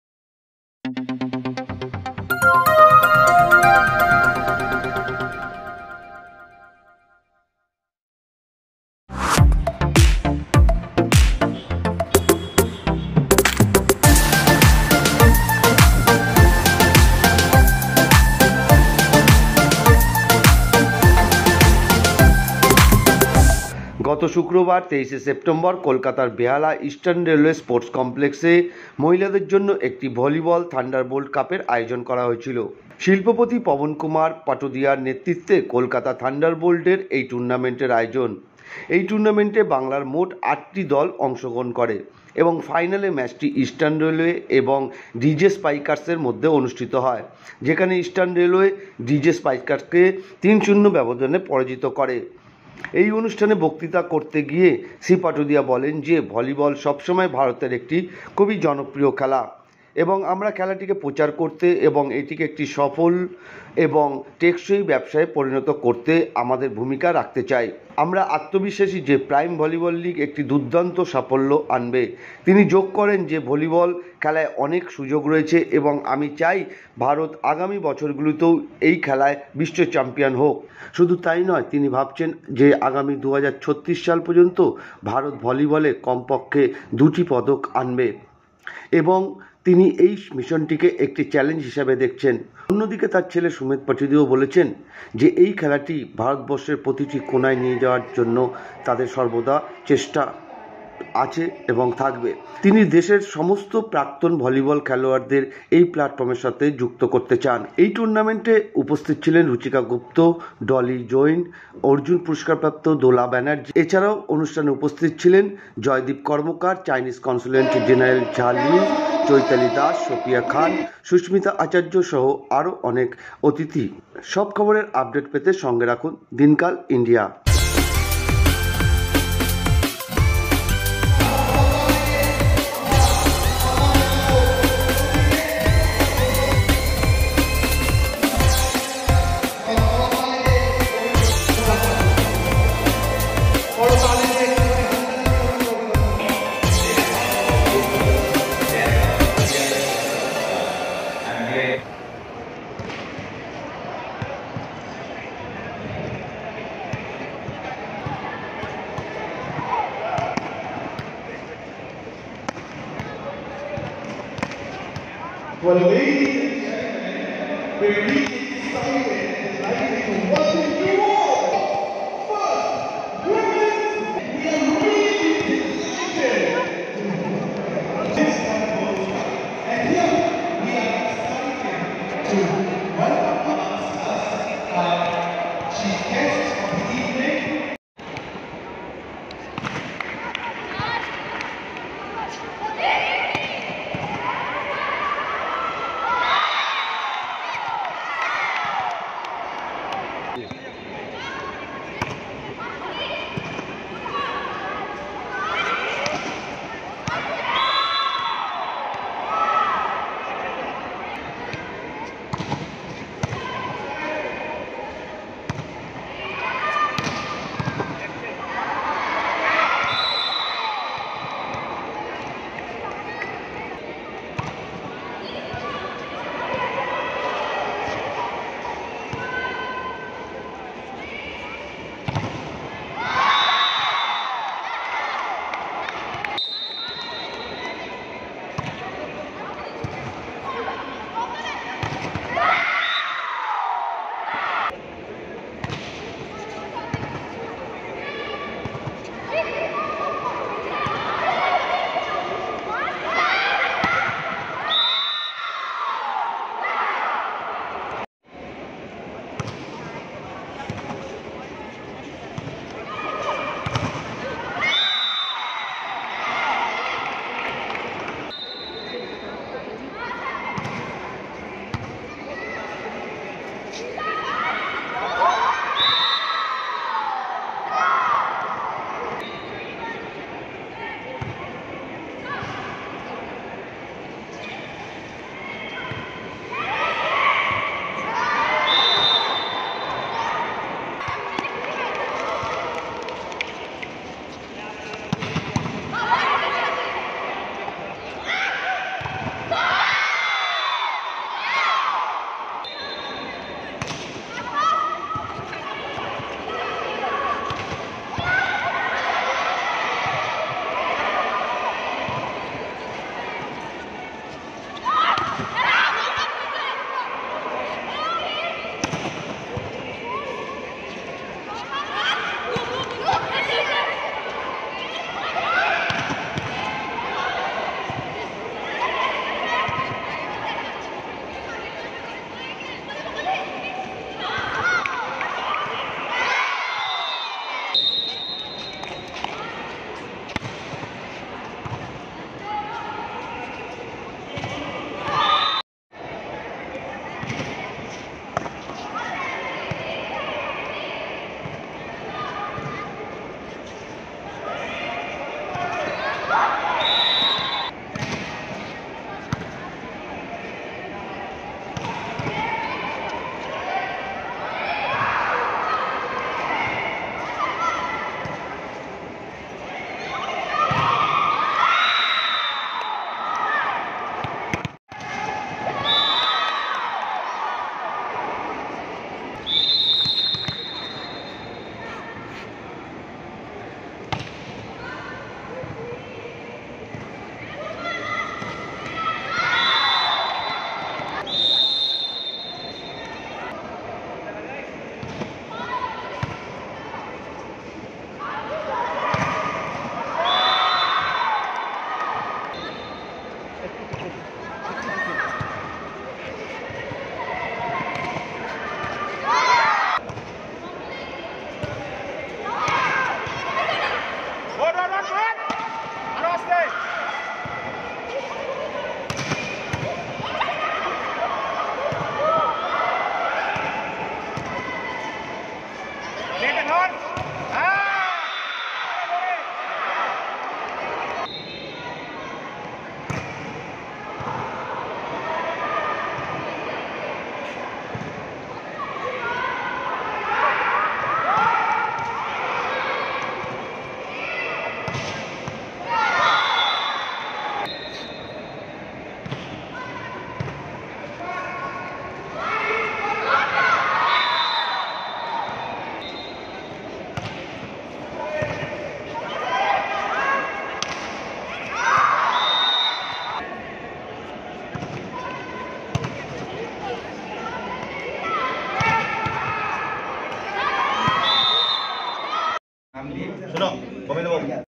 Women's Thunderbolts Cup।বিশ্বের দরবারে ভলিবলকে পৌঁছাতে উদ্যোগ নিলেন শিল্পপতি পবন কুমার পাটোদিয়া
নিজস্ব সংবাদদাতা সৌজন্যে DINKAALINDIA
It is Media Broadcasting Company.For all Type Events, sports,Media Covarege